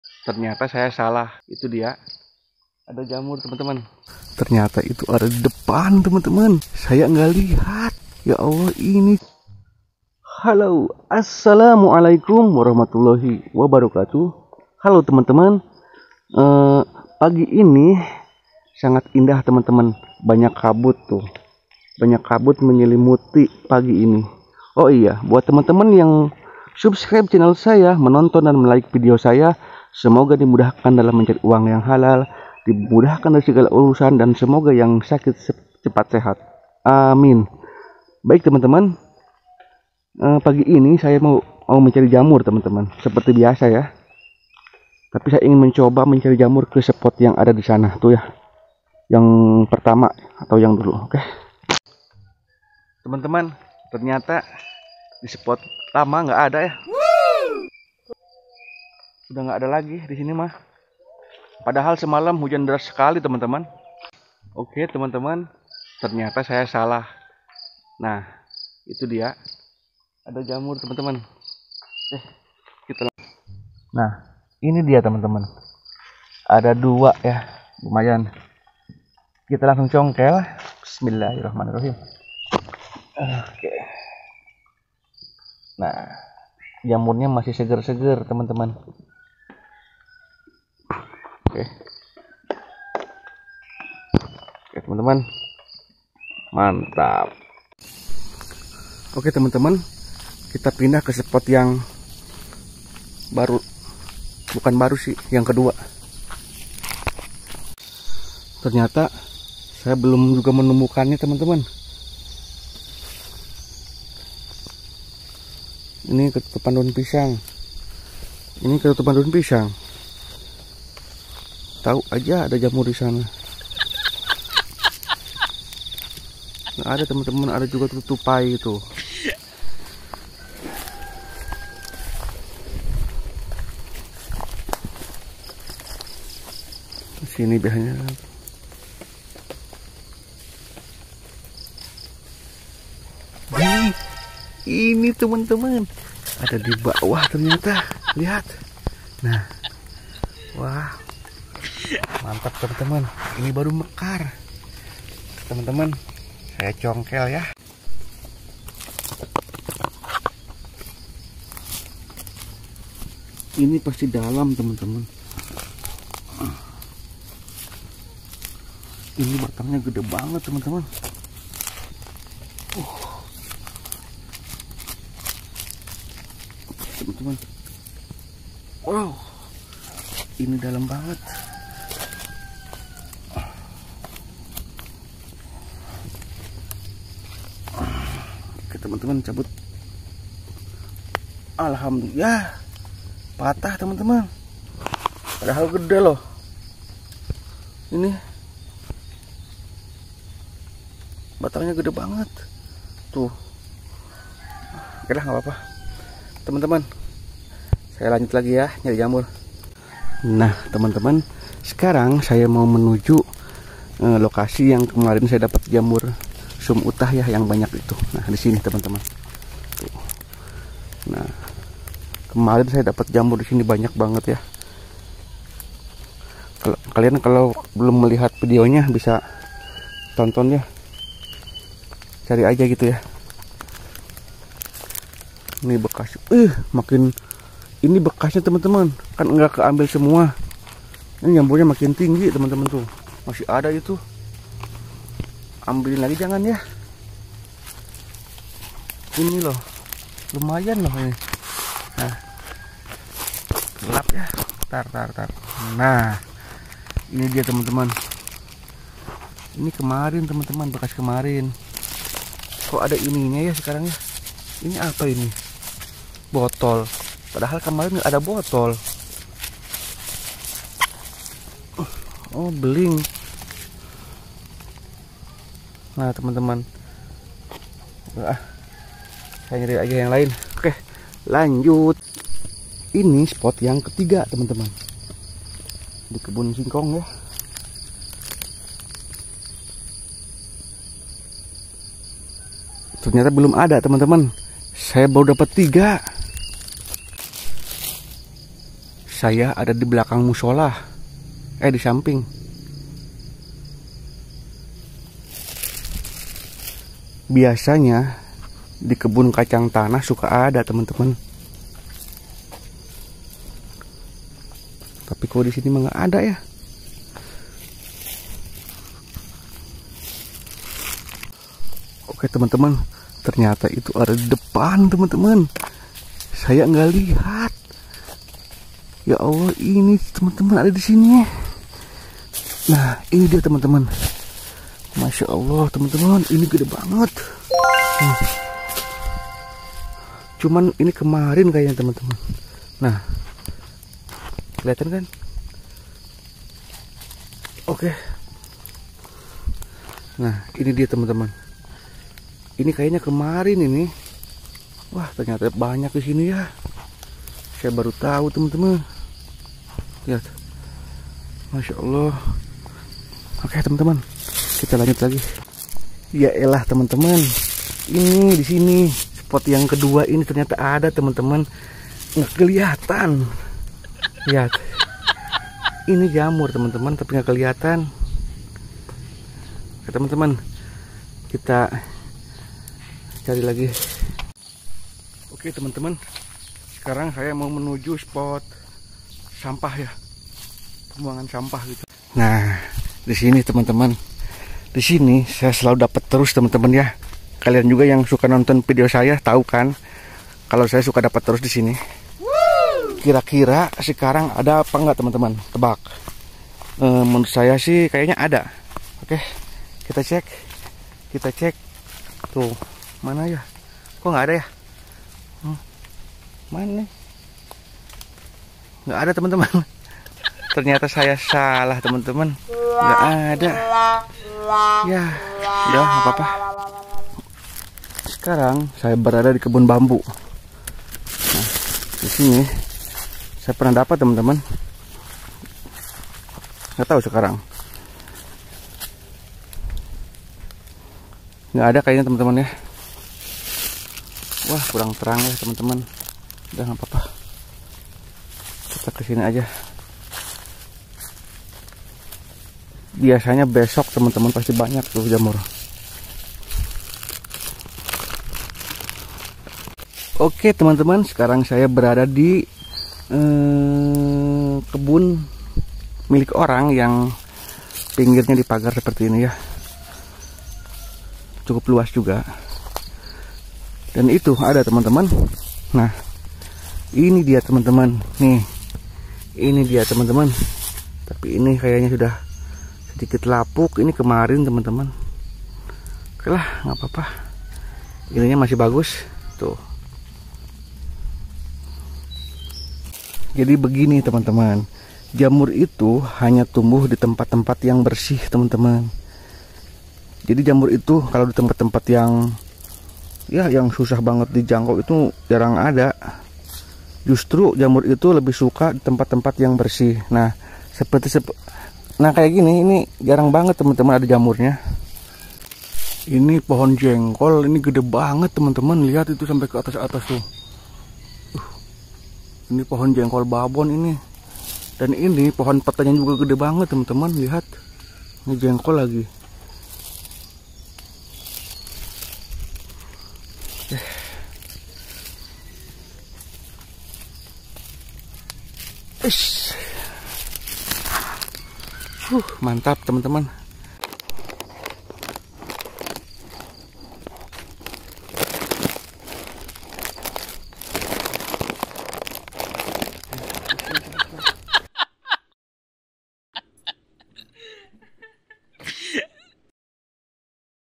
HIDUP DI DESA!!! MUDAHNYA MENCARI JAMUR BARAT TINGGAL KE KEBUN
Alhamdulillah.. saya mendapatkan jamur lagi. Rejeki buat saya dari Alloh SWT. Mencari jamur di pagi hari yang berkabut, sangat menyenangkan sekali.